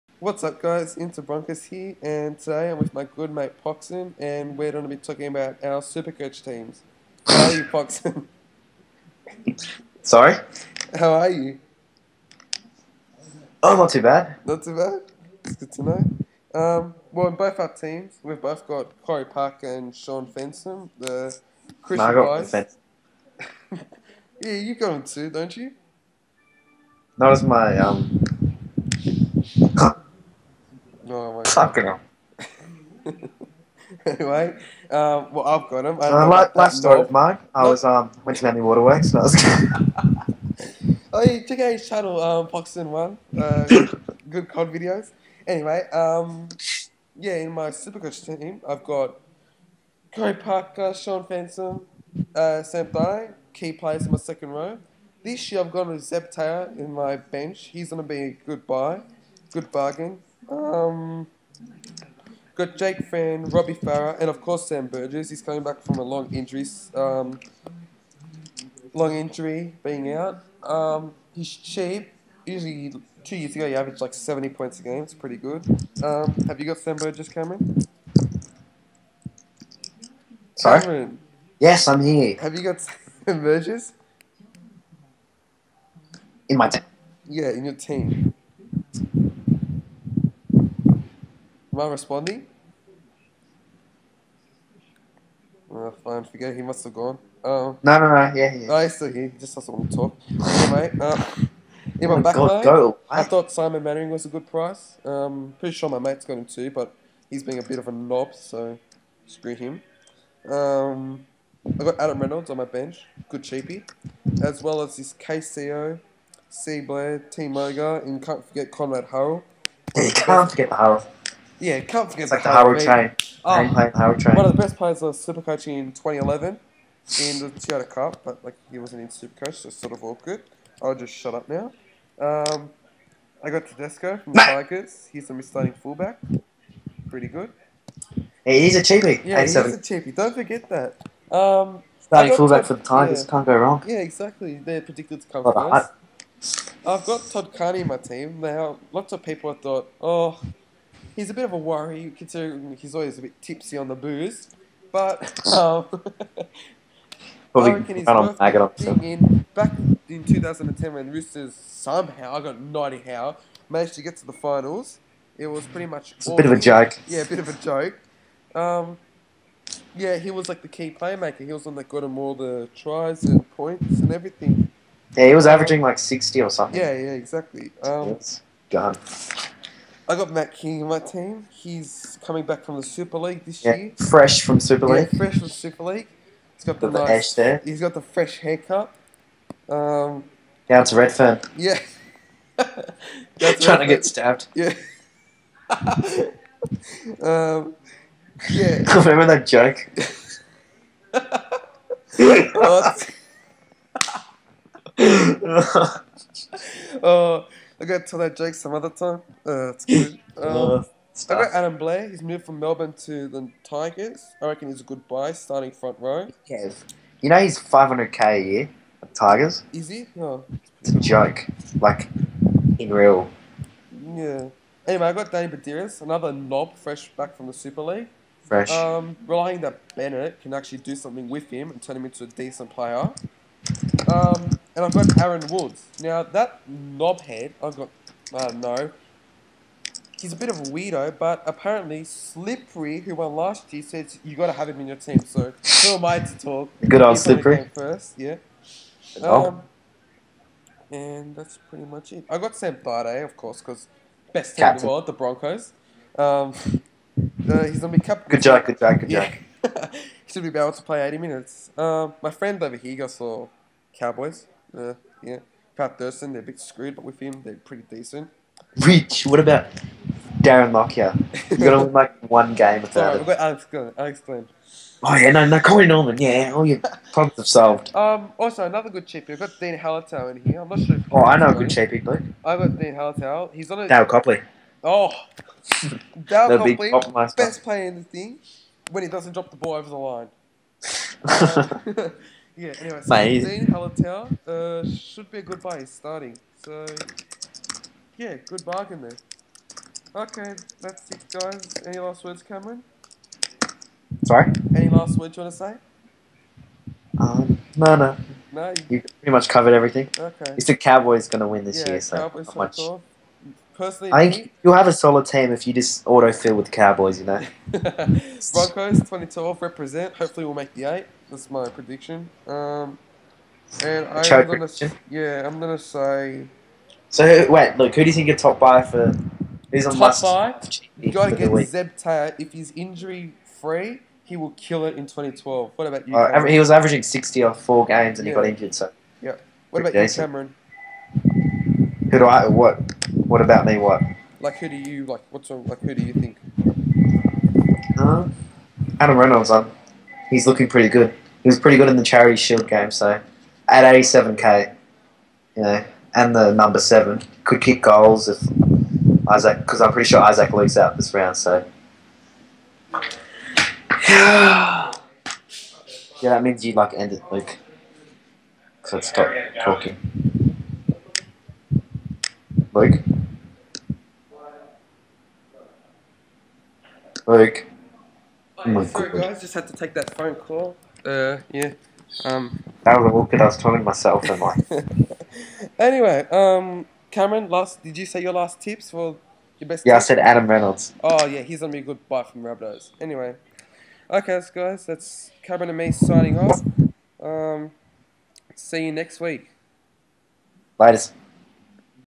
Nrl Supercoach Tips Episode 1
Tips for round 1 of supercoach by interbroncos and Poxn1